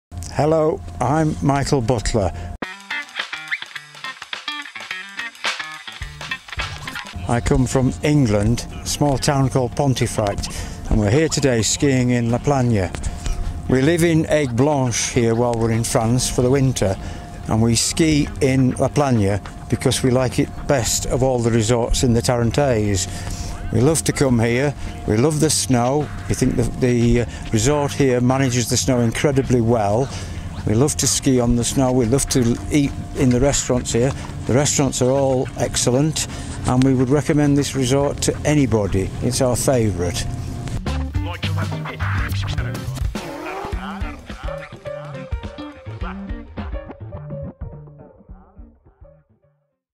Michael was in la Plagne I Michael était à la Plagne
Michael lives in the plagne in winter. He enjoys restaurants and tracks. For him and his wife, it's the best station tarentaise, they recommended it to you!

Michael habite la plagne en hiver. Il aime les restaurants et les pistes. Pour lui et sa femme, c'est la meilleure station tarentaise, ils vous l'ont recommandée !